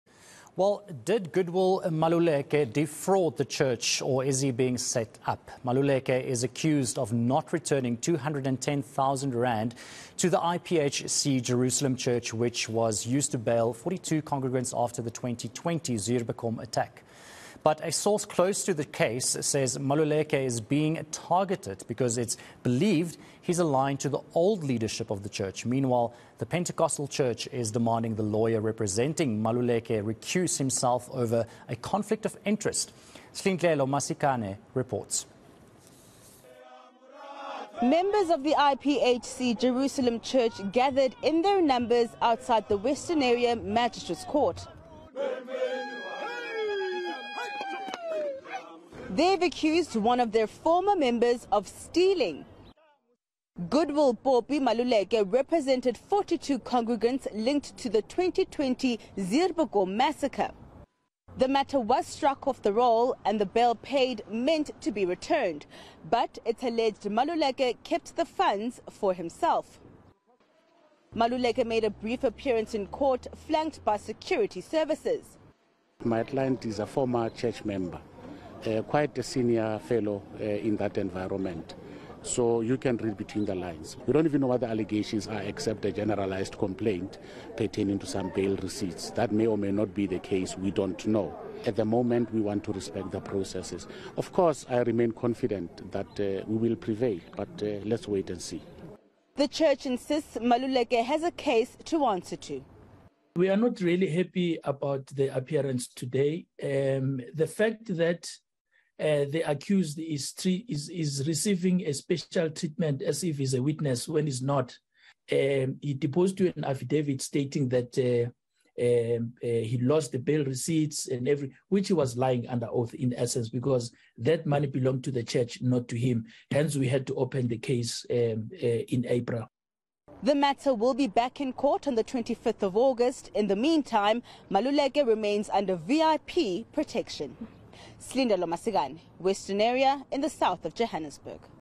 Lawyer accused of defrauding Zuurbekom church
A child has died in a shooting in Midrand, this afternoon. It's believed a woman was picking up her two children when the incident happened. Details are currently sketchy and police are investigating. eNCA's Silindelo Masikane reports. Courtesy